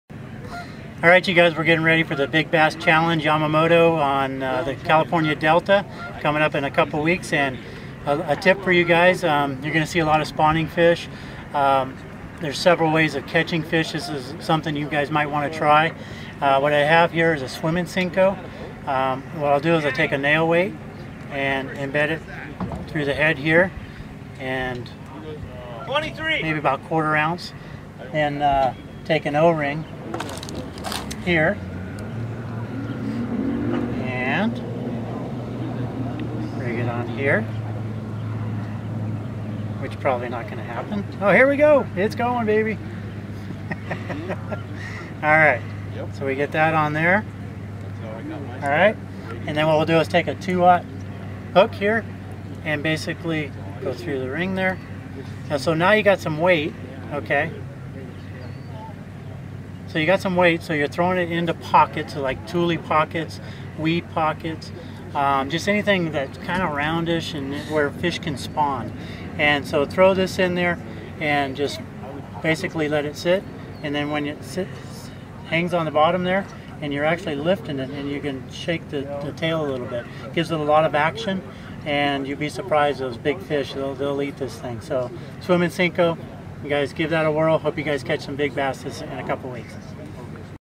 Yamamoto Bait Tips - Jimmy Reese Tip 2
Jimmy Reese gives tips on fishing a Swim Senko.